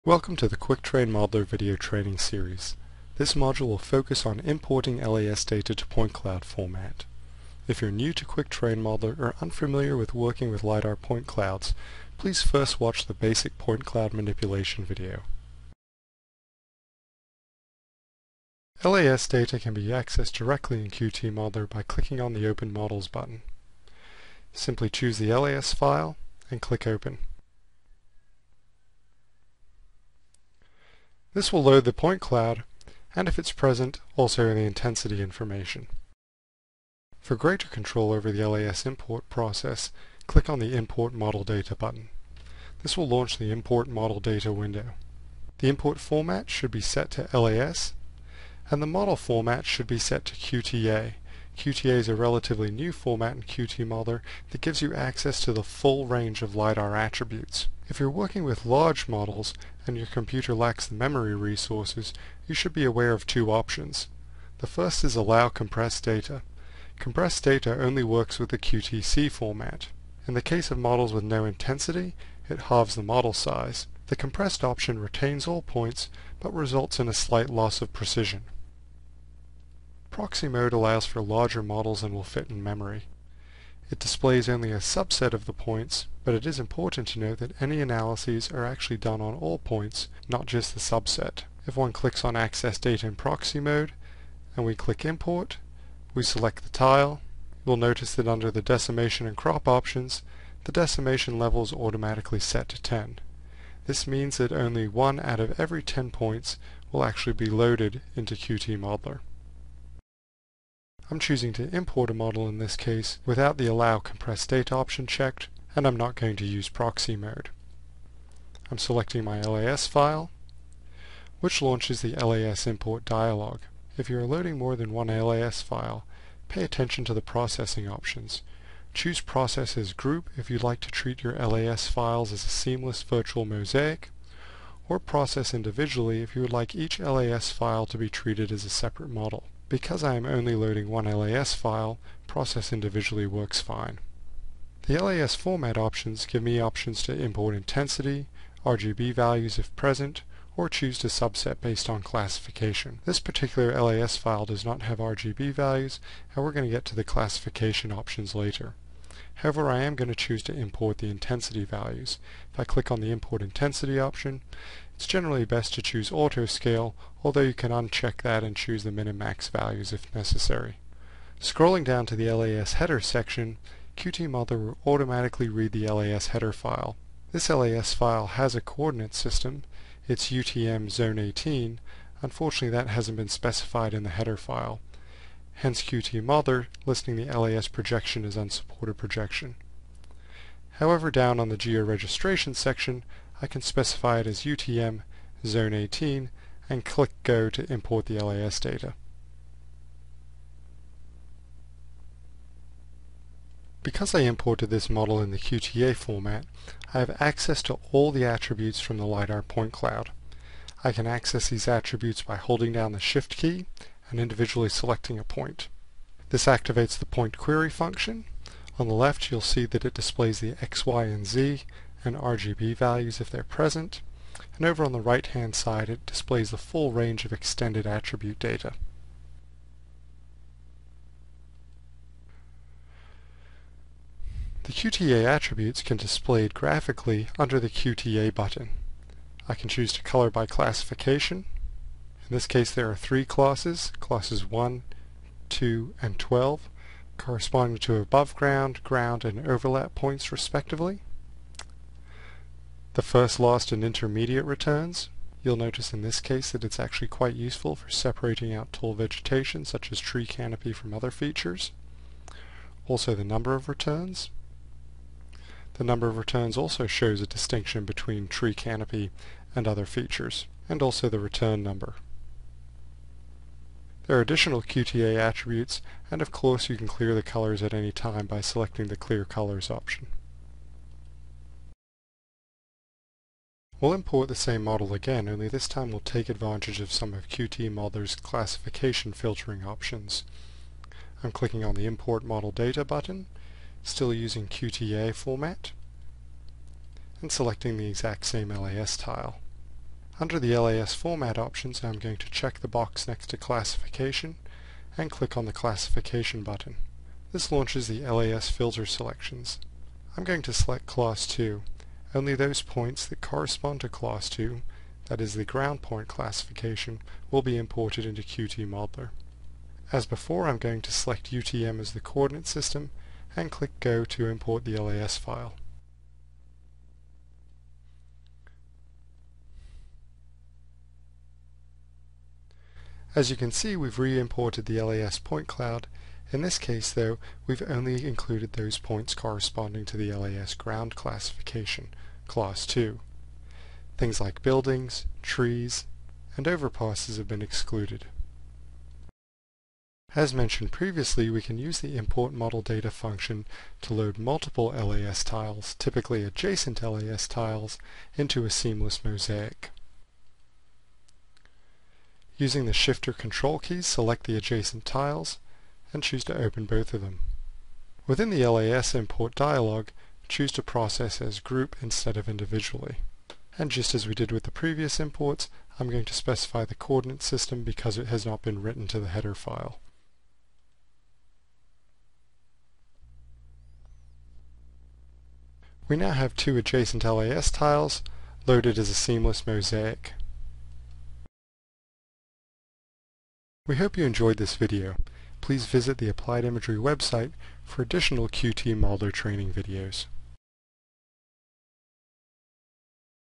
Importing LAS to a Point Cloud - Quick Terrain Modeler
This module covers the basics of creating a point cloud from an LAS file.

Topics Covered:
- Opening LAS files directly (i.e., no import process).
- LAS import window and choices.
- Data compression (an optional process).
- Processing options.
- Format options.
- Georegistration.
- LAS attributes in the point data record and the creation of the QTA attribute file.
- LAS attribute display.
- Point Query - displaying all LAS attributes of a given point.
- Filtering by LAS classification upon import.
- Merging choices: How to either merge LAS data sets into a single point cloud - or keep them separate - upon import.